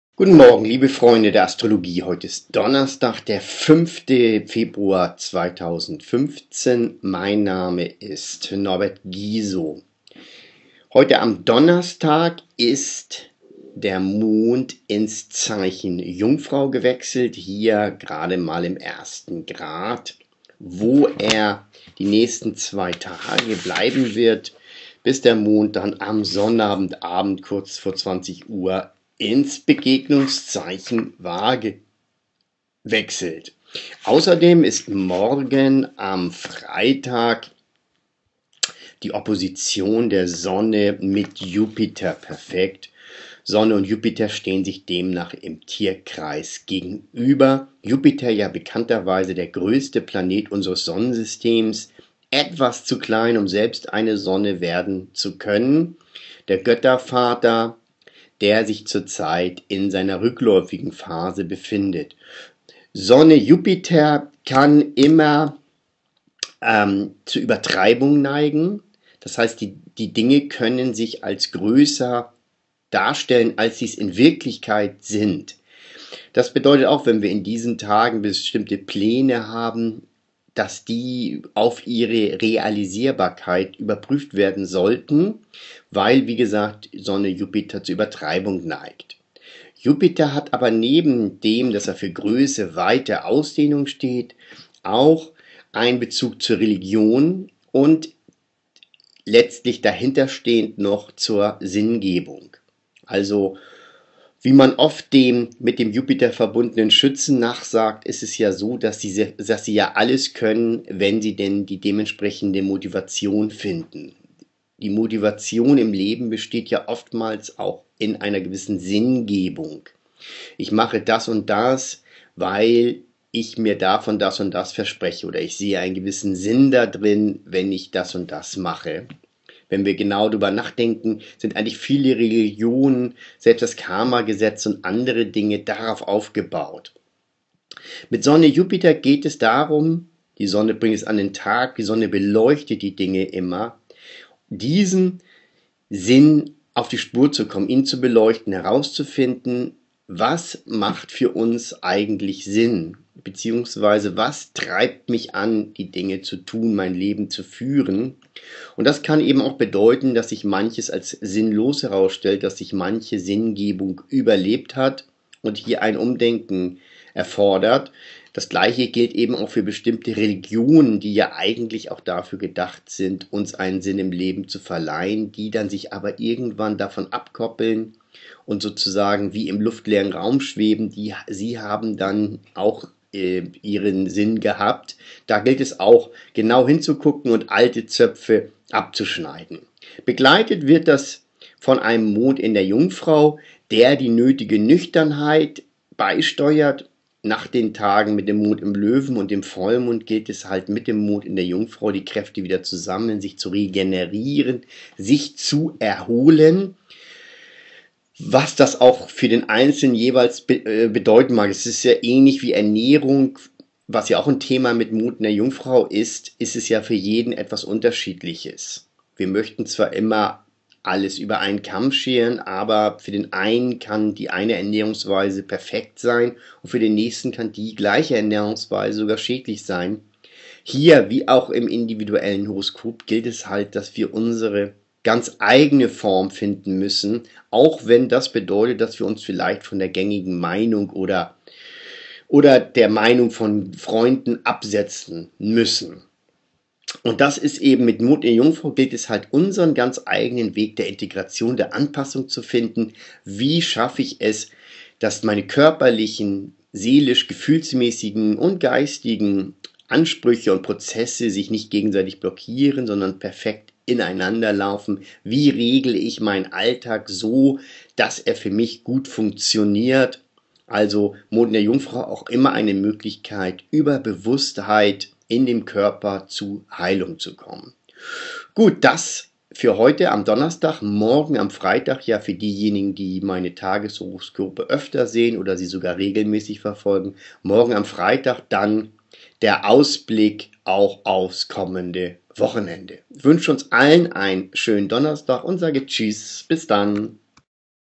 Tageshoroskop Donnerstag 5. Februar 2015
und präsentieren das Tageshoroskop vom 05. Das kann sowohl in negativer wie in positiver Hinsicht der Fall sein. Wir sollten übertriebene Pläne oder Vorstellungen gut überprüfen. Außerdem ist die Sinngebung aktiviert. Was macht für uns in unserem Leben Sinn? Der abnehmende Mond in der Jungfrau legt den Fokus mehr auf den Alltag, auf unseren Körper und die Gesundheit. Achtsamkeit kann zu Heilung führen.